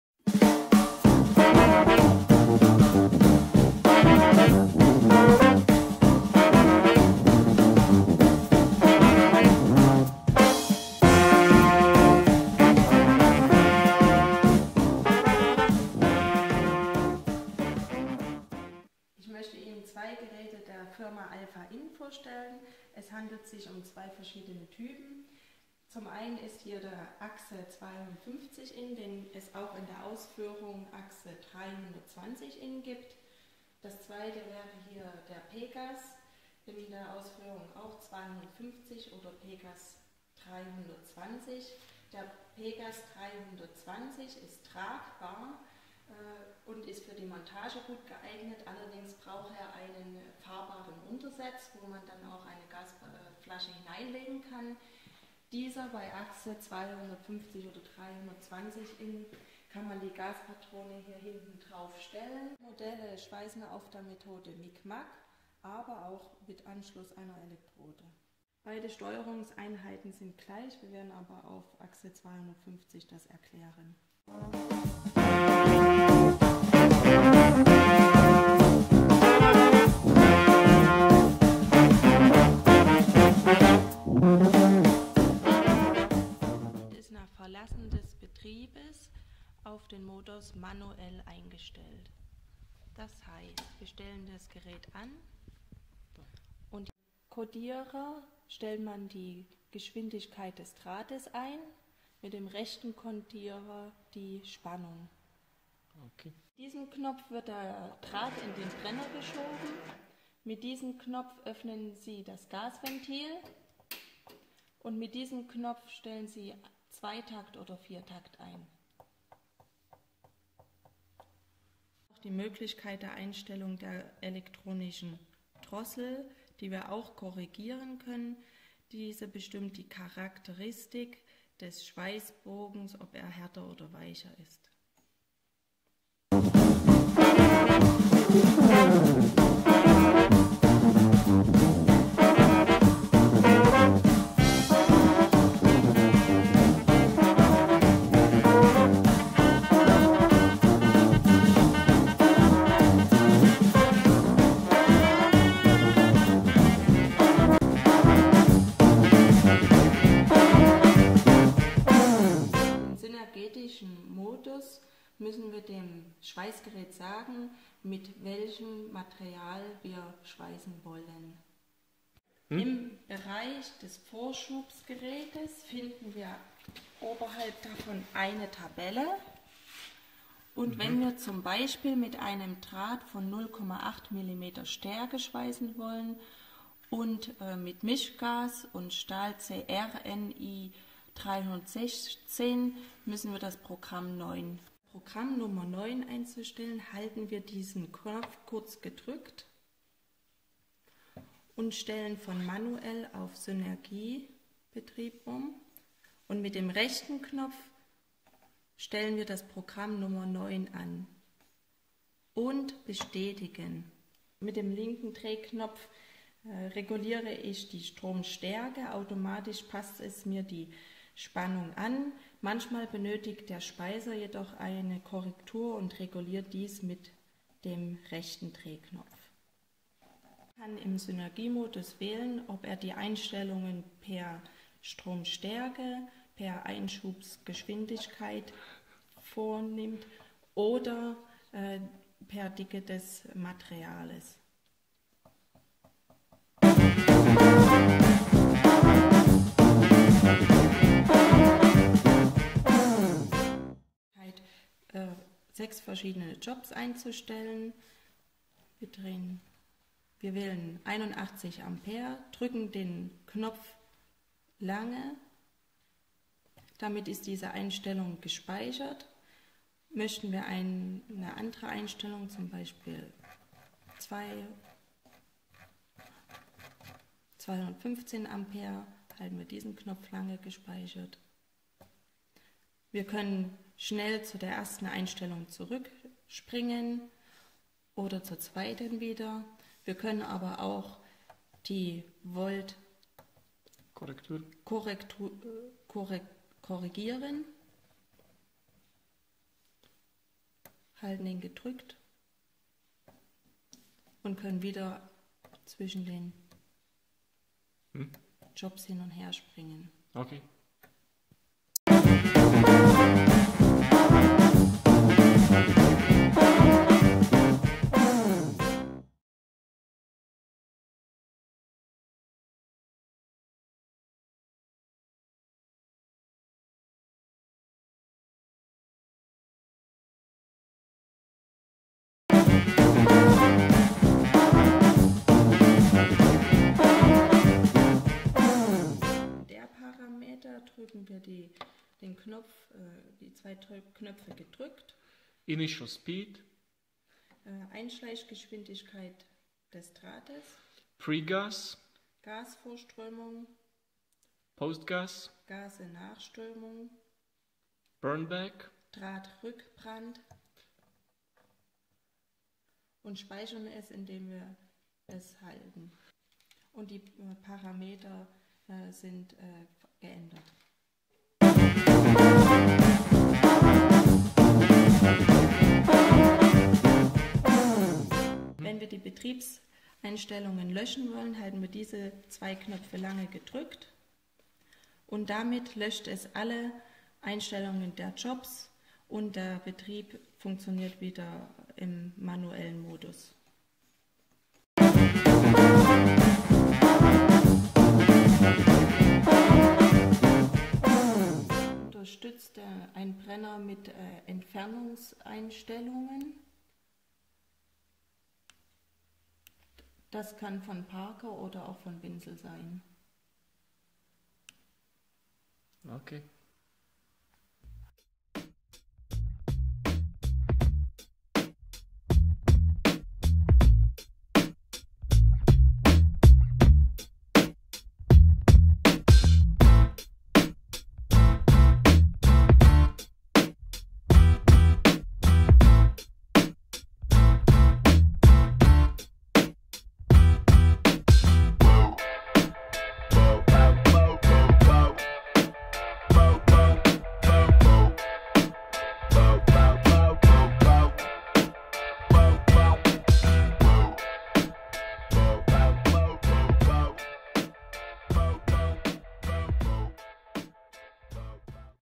aXe PEGAS 250 320 IN Schweißmaschinen (Deutsch)
Liebe Freunde und Partner,
mit Frau Diana drehten wir ein Video über die Bedienungsmöglichkeiten unserer neuen Schweißmaschinen
PEGAS 250/320 MIG SYN und  aXe 250/320 MIG IN SYN.

Uns ist bewußt, dass dieser Film, im Gegensatz zu den  Schweißgeräten, nicht vom „Profi“ kommt. Nichtdestotrotz denken
wir, dass diese Serie Ihre Anerkennung findet und Sie sich beim Anschauen nicht langweilen werden. Vladimir Holy, ALFA IN a.s.. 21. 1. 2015